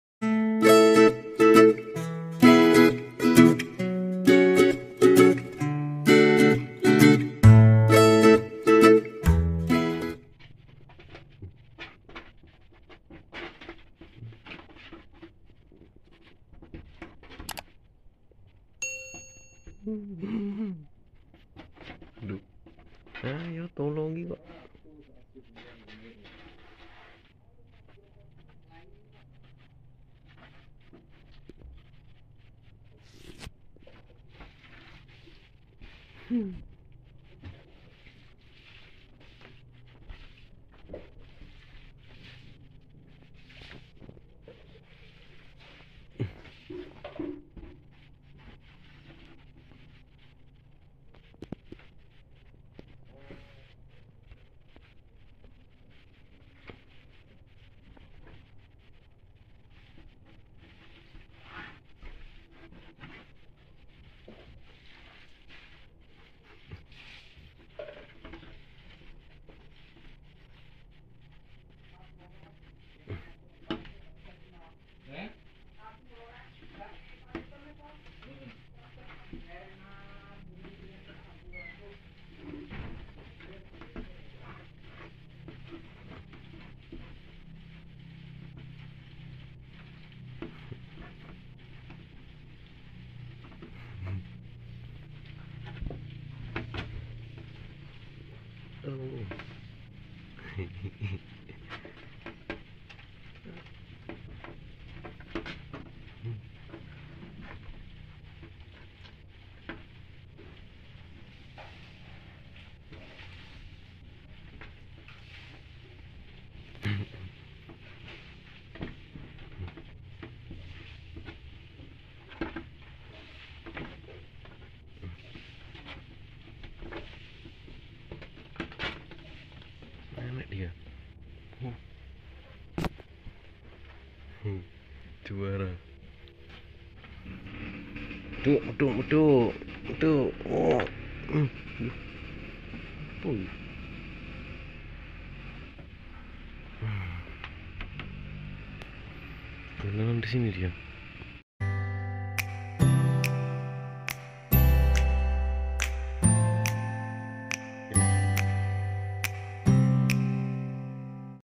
Di tolongin bukanya turun malah ngajak main.. kucing kecil lucu, kucing kecil comel
kucing kecil meong meong minta tolong, pas saya tolongin mau saya turunin eh malah ngajak main 😄

kucing kecil lucu kucing pliharaan saya ..